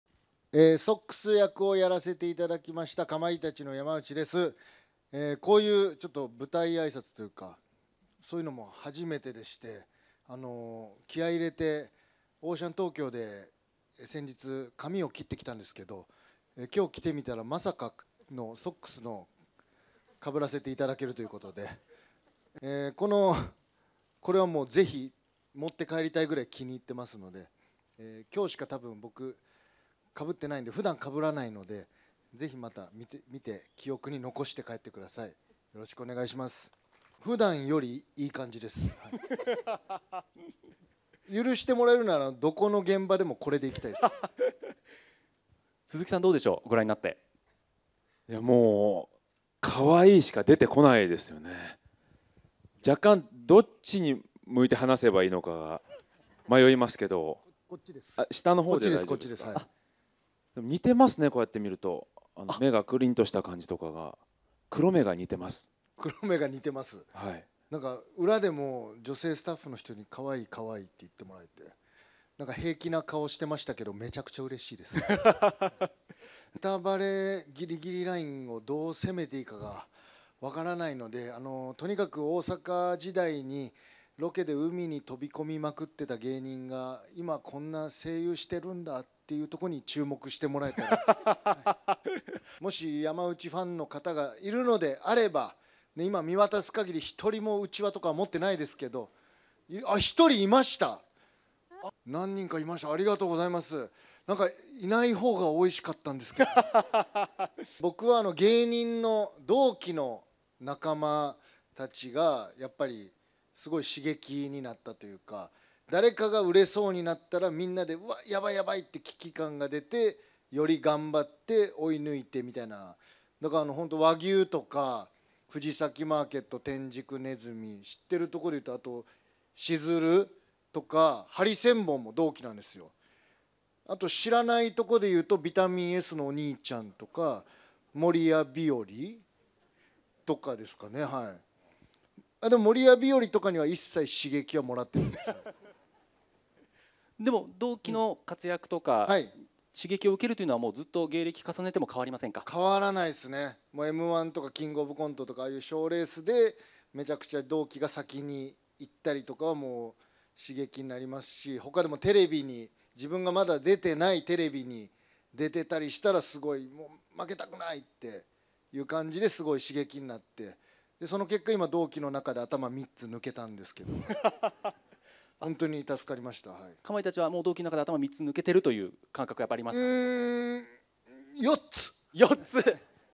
かまいたち山内健司、美容院でオシャレ散髪するも現場では被り物を支給される！ 『バズ・ライトイヤー』初日舞台挨拶
お笑いコンビ・かまいたちの山内健司（41）が1日、都内で行われたディズニー&ピクサー映画『バズ・ライトイヤー』初日舞台あいさつに登壇した。

バズの猫型の友だちロボット・ソックス役の声を担当した山内は、映画の見どころについて「とにかく大阪時代にロケで海に飛び込みまくっていた芸人が、いまこんな声優しているんだというところに注目してもらえれば」と、芸人人生に思いはせながらアピール。

また、人生が変わった仲間との出会いについて話がおよぶと、山内は「同期の仲間たちがすごい刺激になった」と打ち明けた。和牛、藤崎マーケット、しずる、ハリセンボンなどの名前を挙げ「同期の誰かが売れそうになったら、みんなでやばいと危機感があって、より頑張って追い抜いてという感じだった」と振り返った。

芸歴を重ねても同期の活躍には刺激を受けるといい、「『M-1』とか『キングオブコント』とか賞レースで同期が先に行ったりとか、自分が出ていないテレビに出ていたりしたら負けたくないって刺激になって、その結果、同期の中で頭3つ抜けたんですけど」と冗談めかせた。さらに、「かまいたちは頭3つ抜けている感覚はあるか？」と再確認されると、「んー…4つ！　4つにしておきます」と笑いを誘った。

映画『トイ・ストーリー』シリーズ最新作。大人気キャラクター“バズ・ライトイヤー”は、少年アンディのお気に入りのおもちゃだが、実は、アンディが人生を変えられるほど夢中になって観ていた映画の主人公でもある。今作は、そんなアンディ少年が大好きだったスペース・レンジャーとして活躍するバズの姿を描いたオリジンストーリー。

ほかに、声優を務めた俳優の鈴木亮平（39）、今田美桜（25）、りょう（49）が出席した。